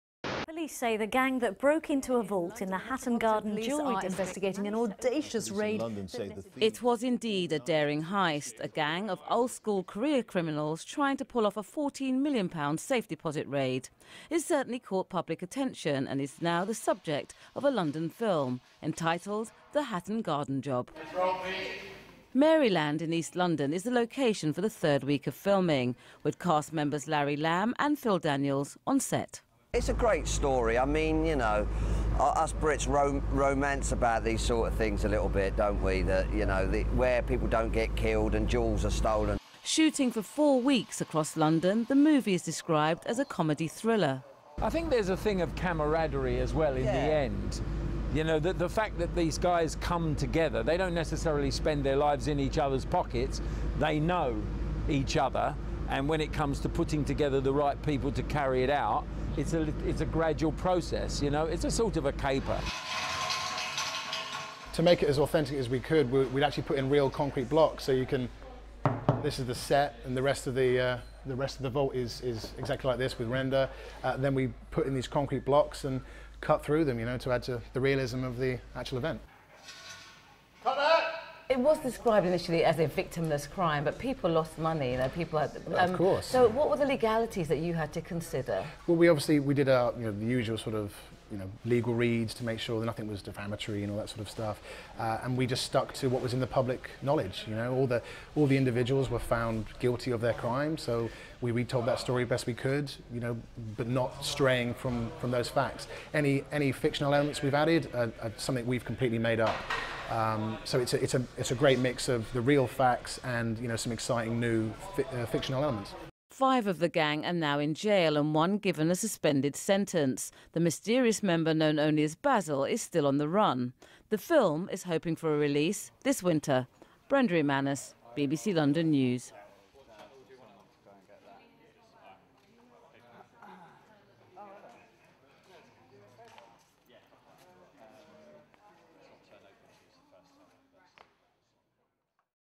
Behind the scenes of The Hatton Garden Job film
It was as audacious as it was unbelievable. Old time criminals going for their last big job - a heist on Hatton Garden during the Easter Weekend in 2015 - the biggest burglary in English legal history.

Hardly surprising then that it's the subject of a new film - being shot in the London.

Our Entertainment Correspondent Brenda Emmanus has been on set in east London with some recognisable faces.